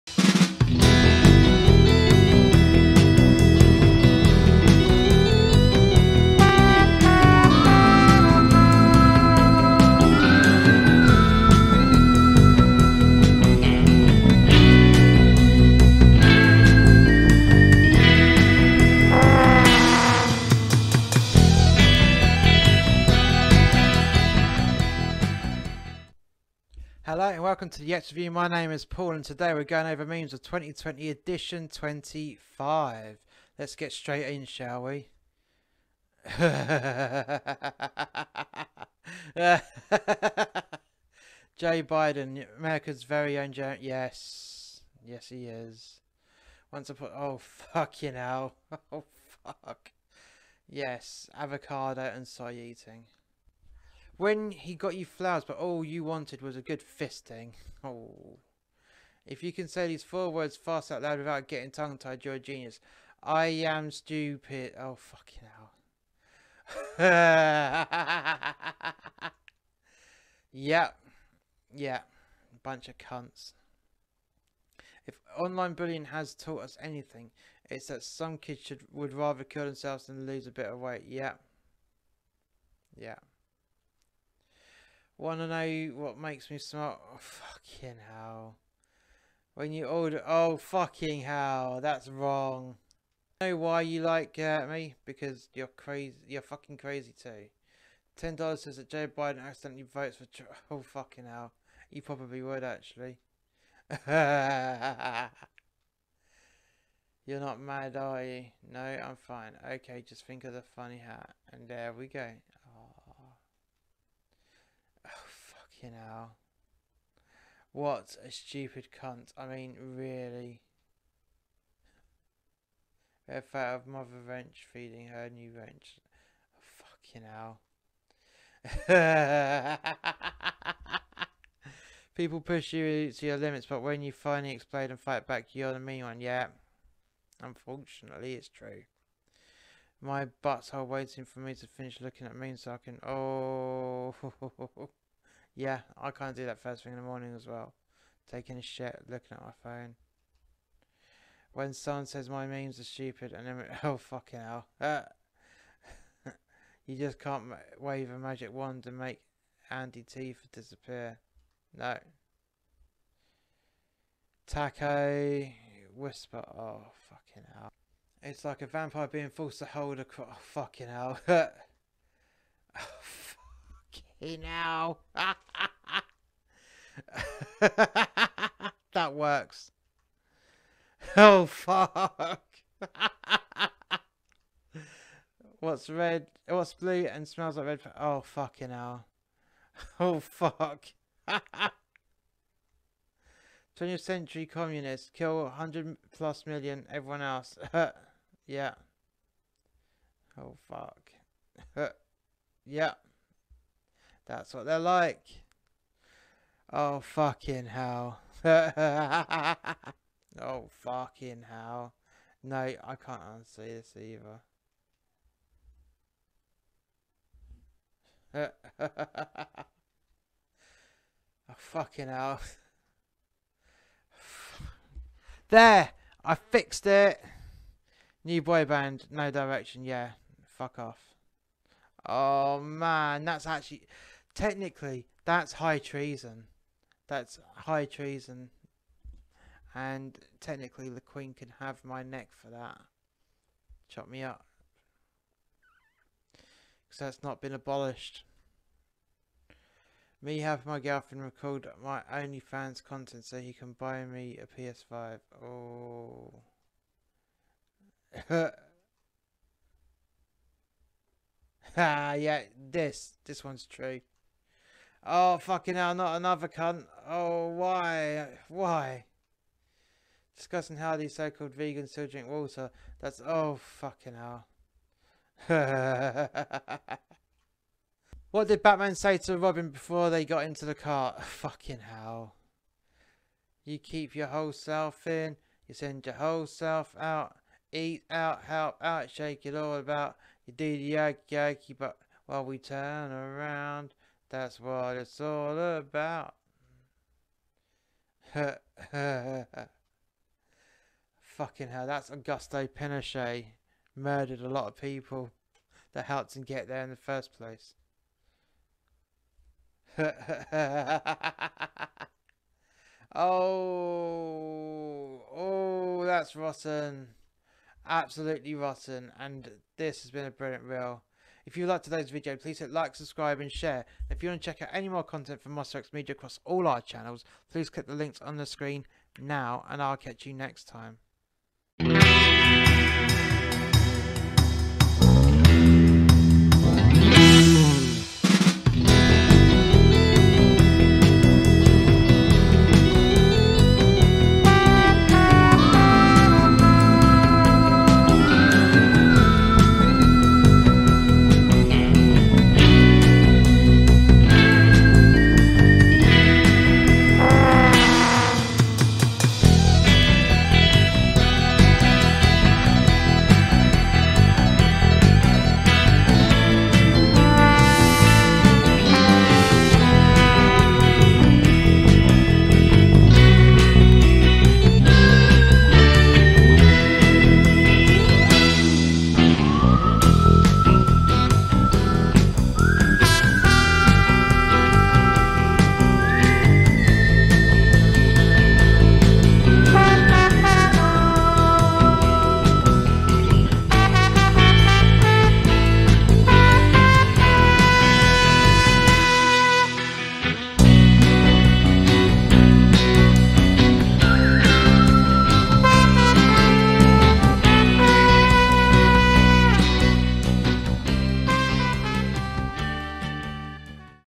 Review and Reaction: Memes Of 2020 #25
Video 7 of New years, Paul takes you over another selection of memes from 2020. Have you seen any?